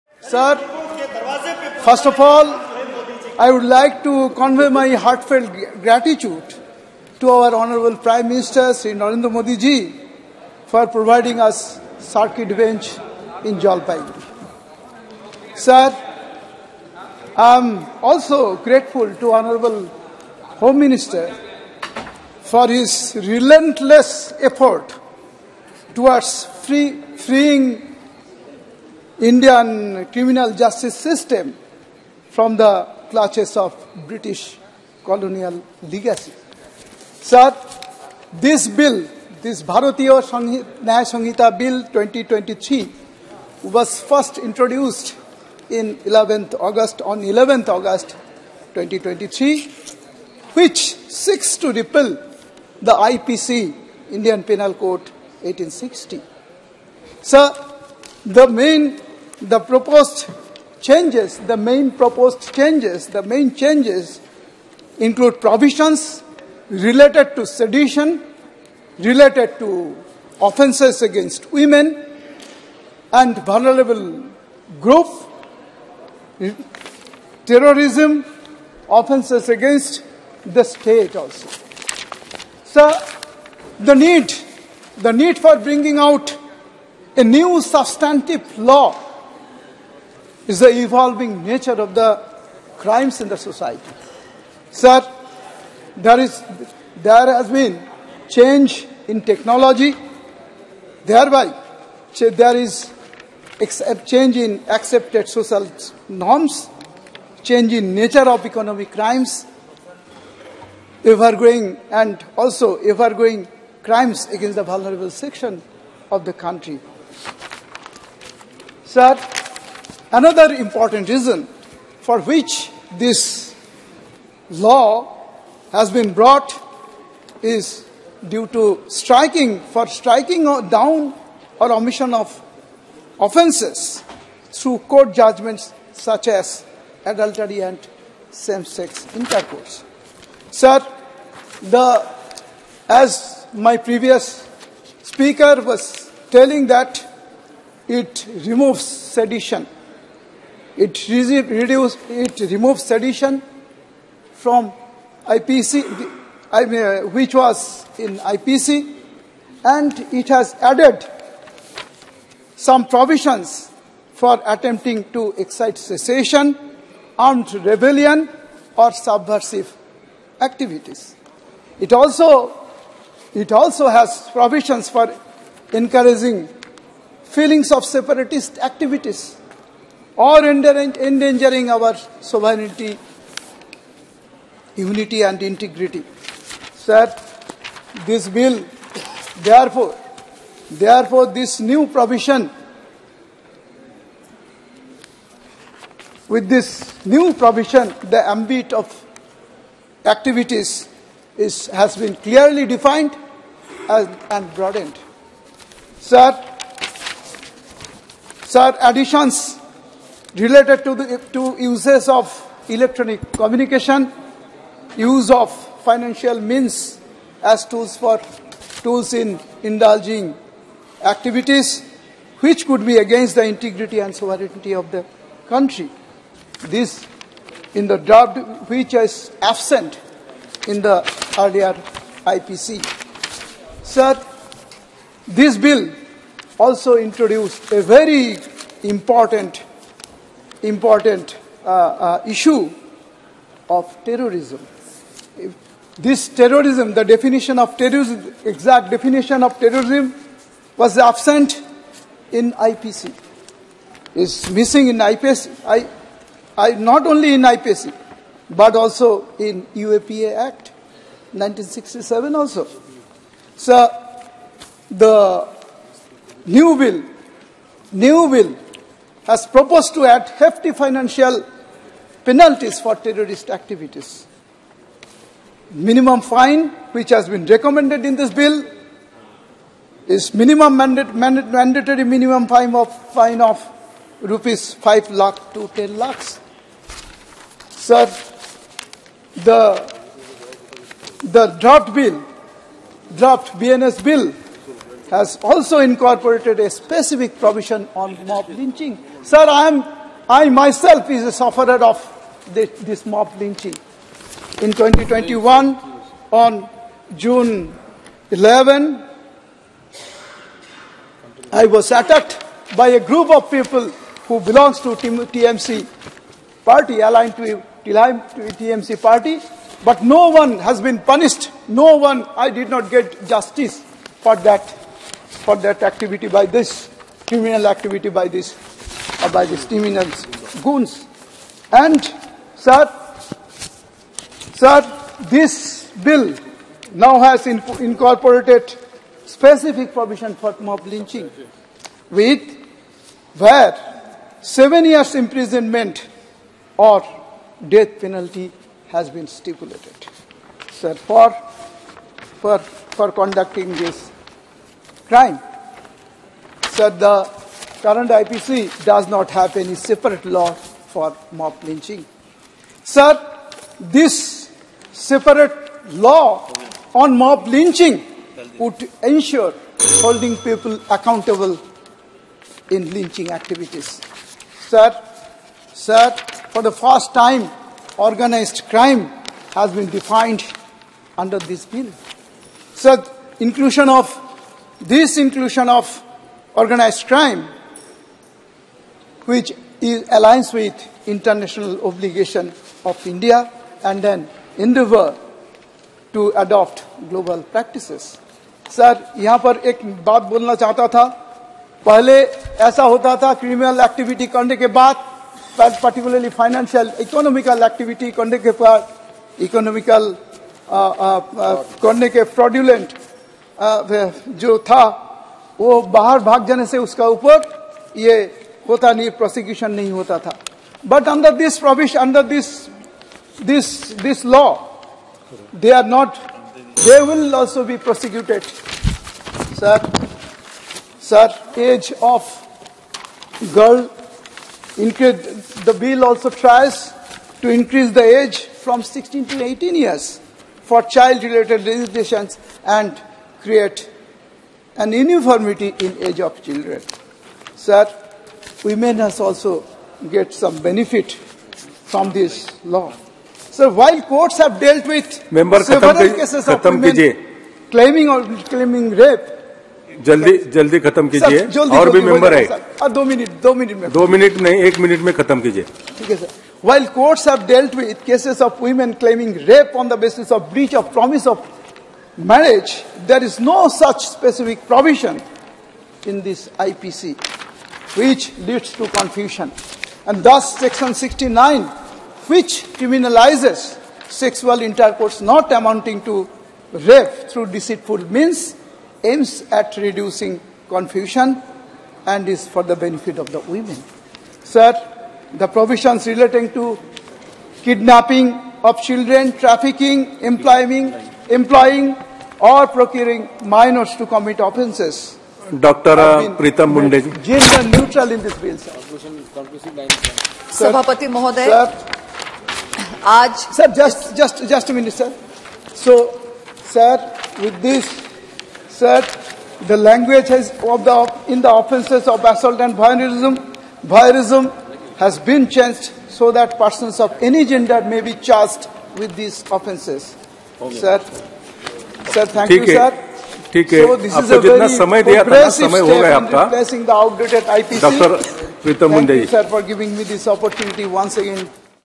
Dr. Jayanta Kumar Roy on BN (2nd)Sanhita, NS (2nd) Sanhita & Sakshya (2nd) Bills, 2023.
Dr. Jayanta Kumar Roy on BN (2nd)Sanhita, NS (2nd) Sanhita & Sakshya (2nd) Bills, 2023 in Lok Sabha: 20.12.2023